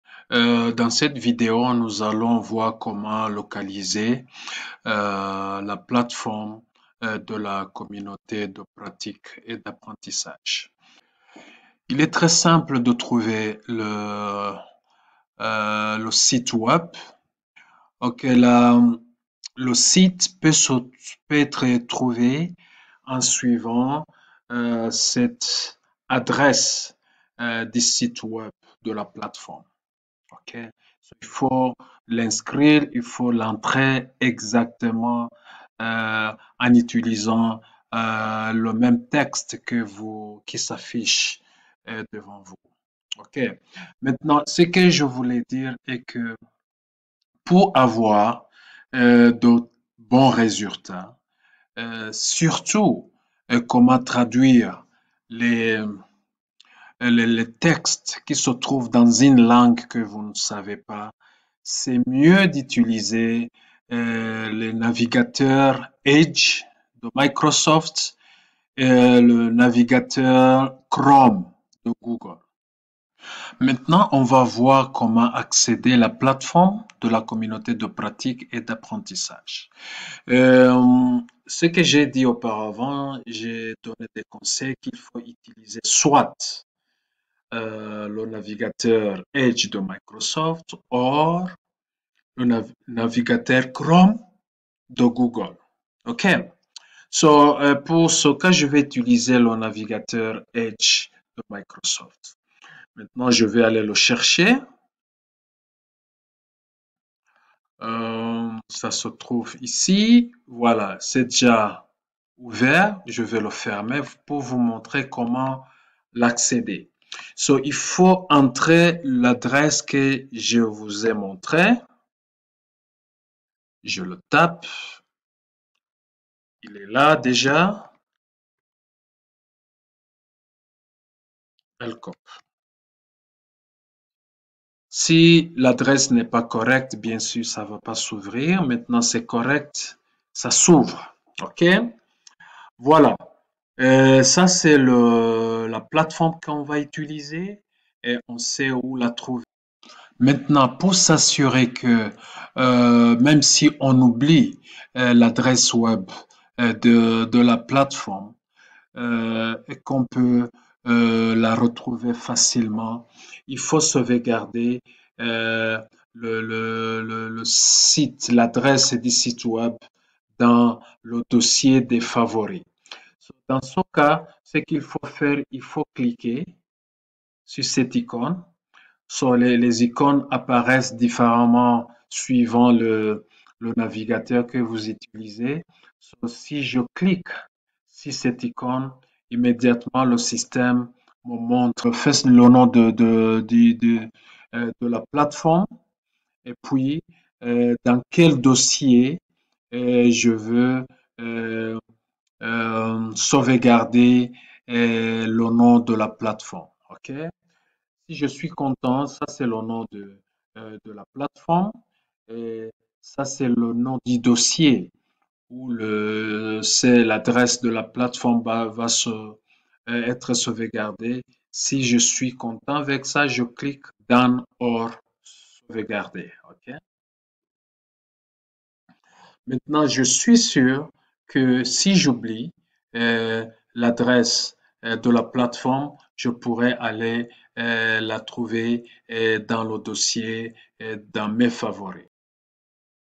Vidéo3 Comment trouver la plateforme LCoP
Cette vidéo montre où trouver la plateforme LCoP.